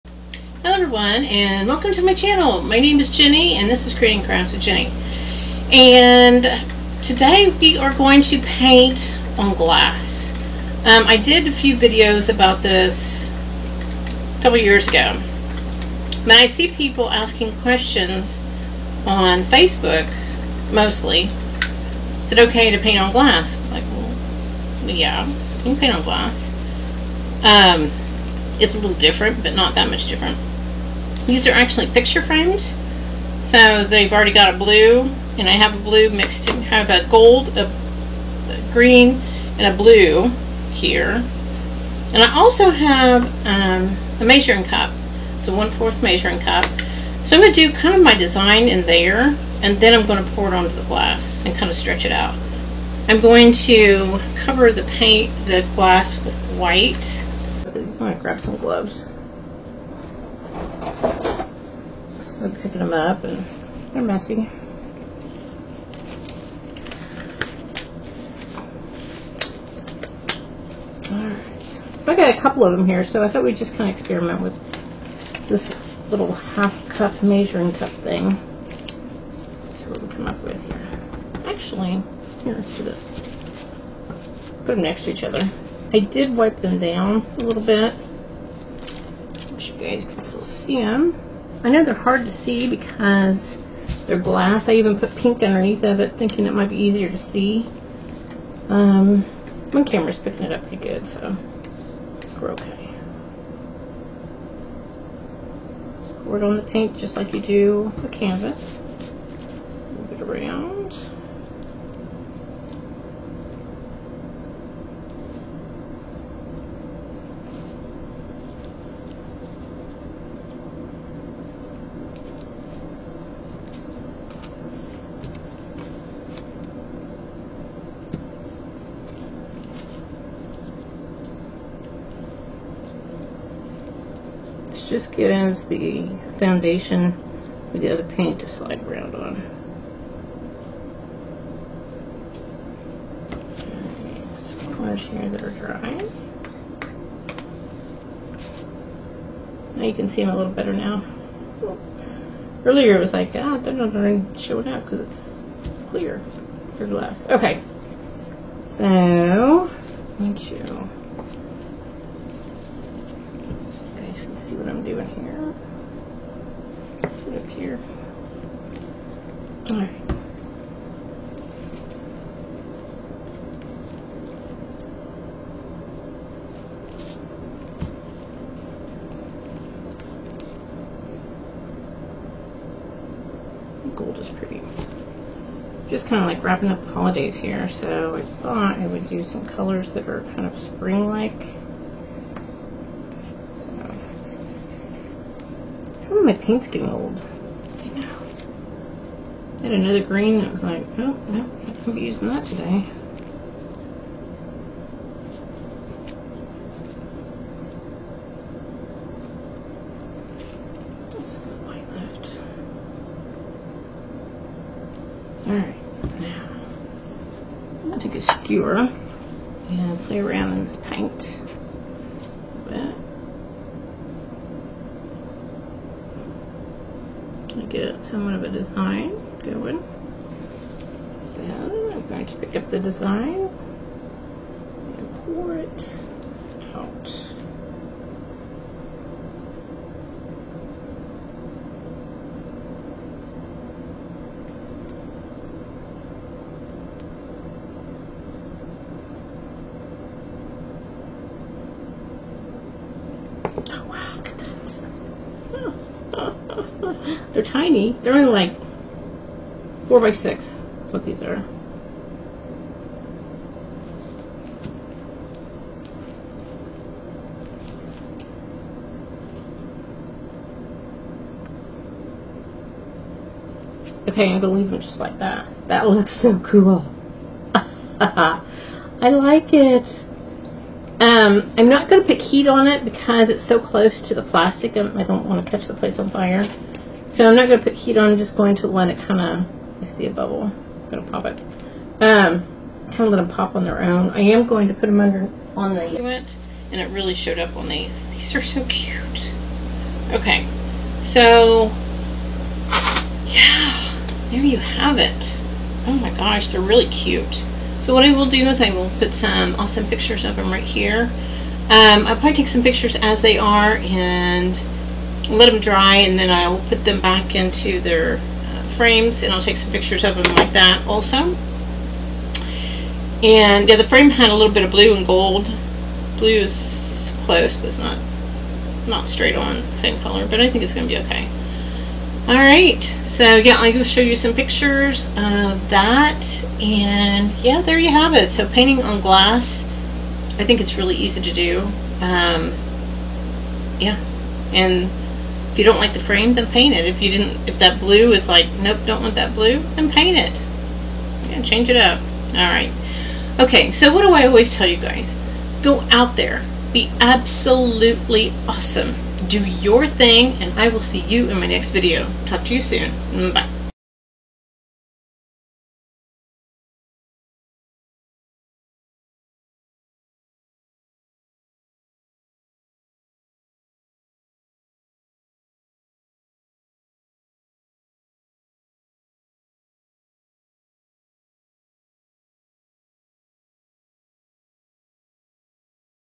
Pouring on glass, love it!!
Art, painting, and crafts, oh my. What a fantastic combination to learn how to create beautiful art on a budget. Or how to create beautiful acrylic pours on a smaller canvas? I absolutely love to be creative.